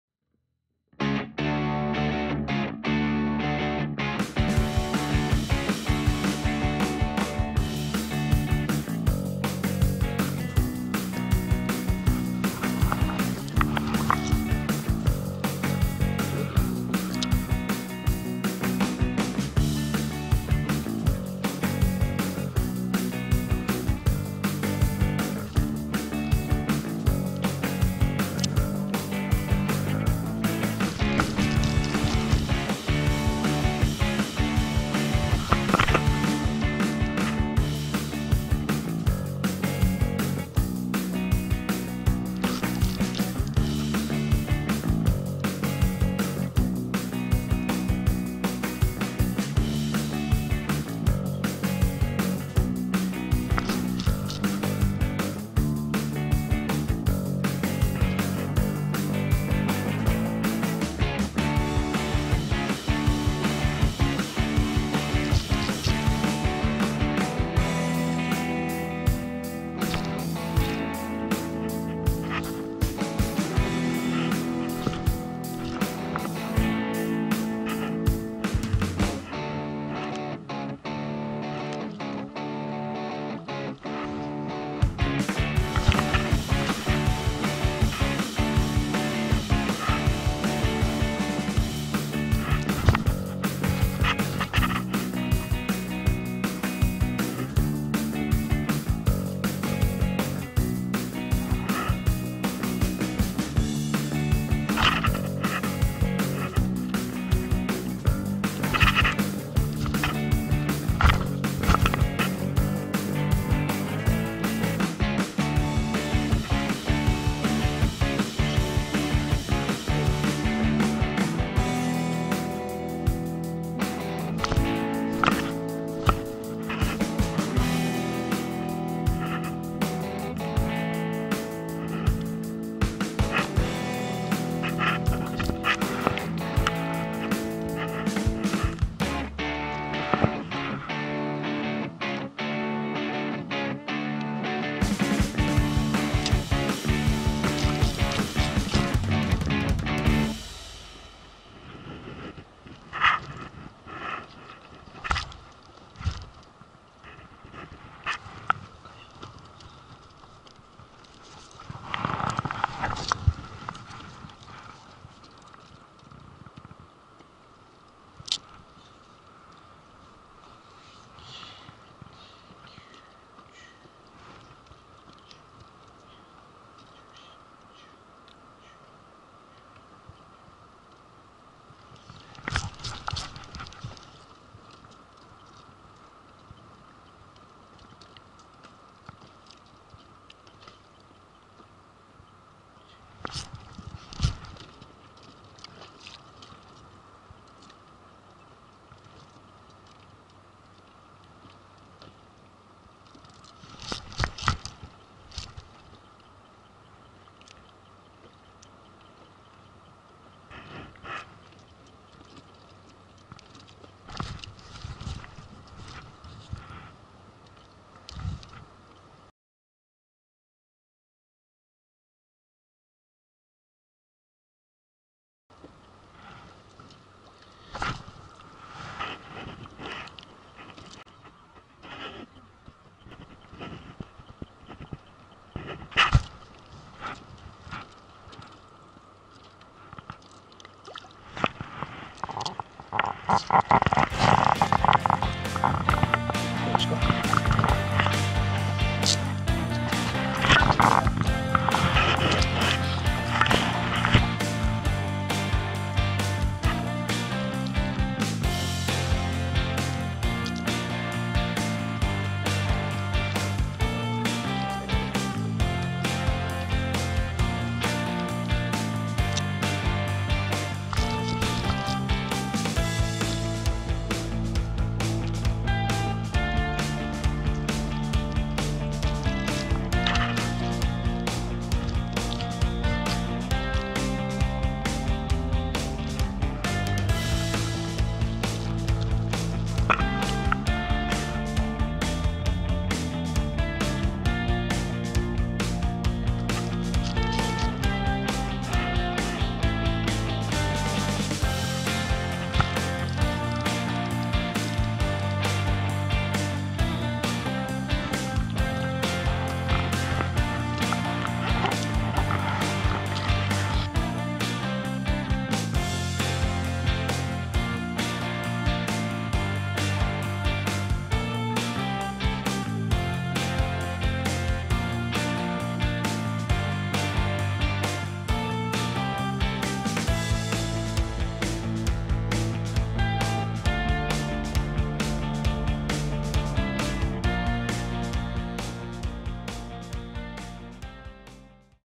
ボートエギングで秋の子烏賊狙い！サイトフィッシングでキャッチ成功！！
そろそろ秋のシーズンを迎えたアオリイカ。
この季節は、まだスレていない子烏賊なので・・・
素人の自分にも、そこそこ釣れるはず。

期待を込めてエギをキャスト！
シャクリながら誘いあげてくると・・・

エギの後ろを小さなイカが追いかけてきます。
焦らしたり誘ったりしつつ・・・
サイトフィッシング！